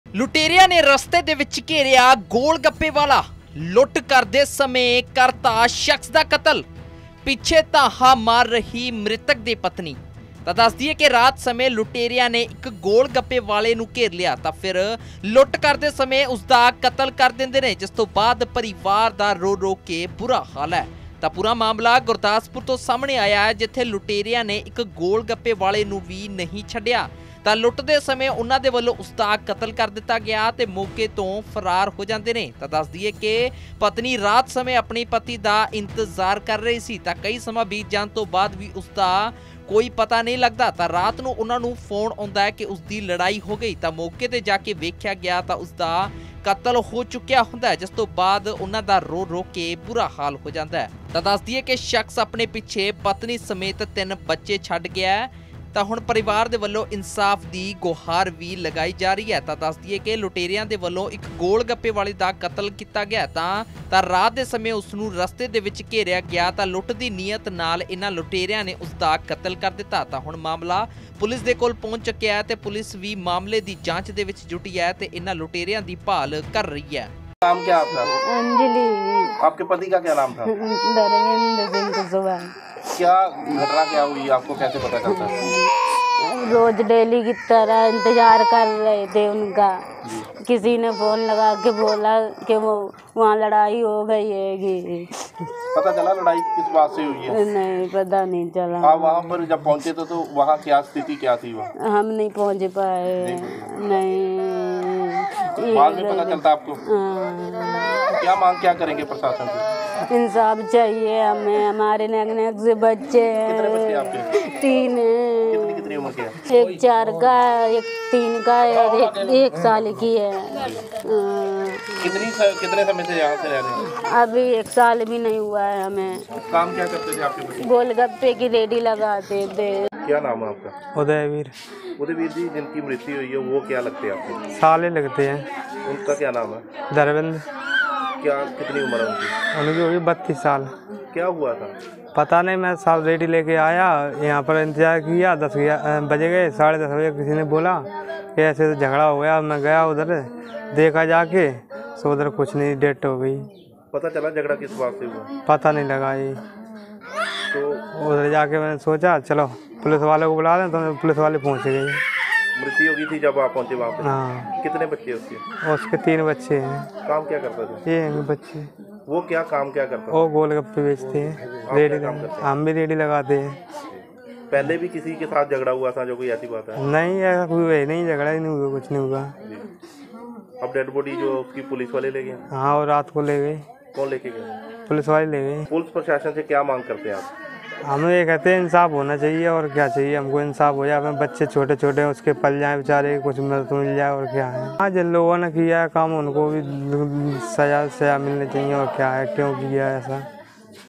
Gurdaspur Murder | ਗੋਲਗੱਪੇ ਵਾਲੇ ਨੂੰ ਵੀ ਨਹੀਂ ਛੱਡਿਆ ਲੁਟੇਰਿਆਂ ਨੇ, ਲੁੱਟ ਸਮੇਂ ਕਰ ਤਾ ਕਤਲ
ਗੁਰਦਾਸਪੁਰ ਦੇ ਕਸਬਾ ਭੈਣੀ ਮੀਆਂ ਖਾਂ ਵਿੱਚ ਦੇਰ ਰਾਤ ਗੋਲਗੱਪੇ ਦੀ ਰੇਹੜੀ ਲਗਾ ਕੇ ਘਰ ਵਾਪਿਸ ਆ ਰਹੇ ਇਕ ਪ੍ਰਵਾਸੀ ਦਾ ਕਤਲ ਹੋਣ ਦੀ ਖਬਰ ਸਾਹਮਣੇ ਆਈ ਹੈ।